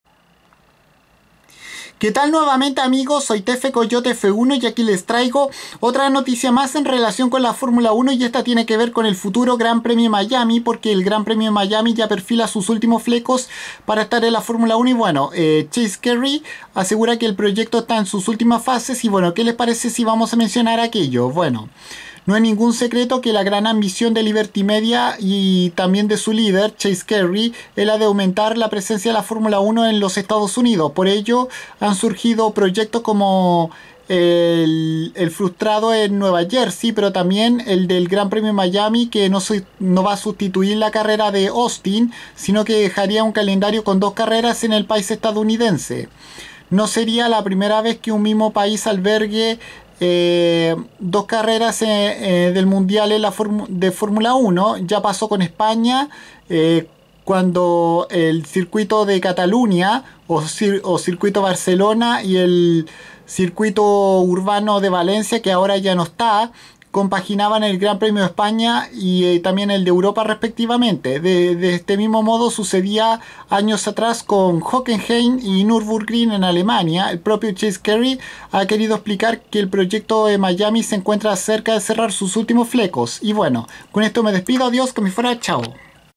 El GP de Miami afina sus últimos detalles para debutar en Fórmula 1
Sería la segunda carrera de la F1 que disputa en suelo estadounidense después del ya existente GP de Estados Unidos en Austin Texas el la pista Circuit of the Americas